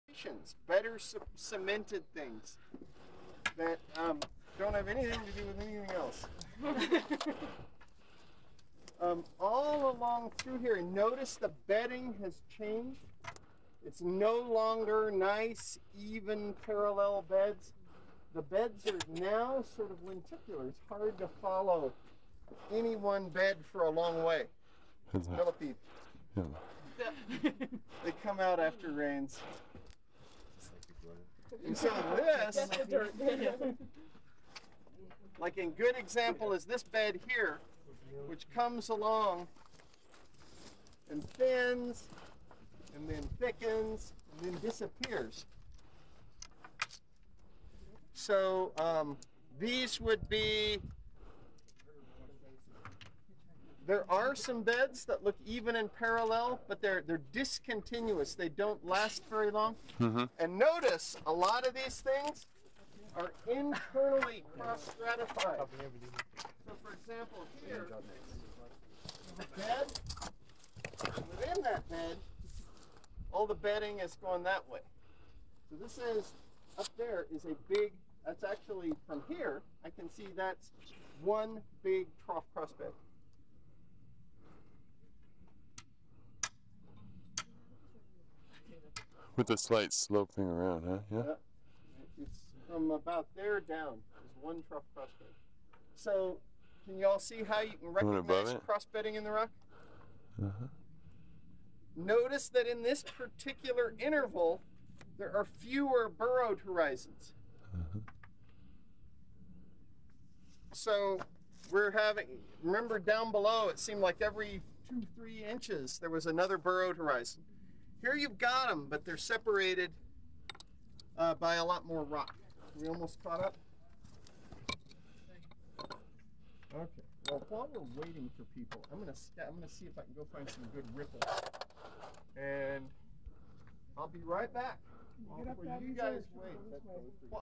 Mckelligon Canyon (pt.8)
Field Trip to McKelligon Canyon 09/05/2014
Sedimentology and Structure (UTEP)
Segment 8
direct from glass no edit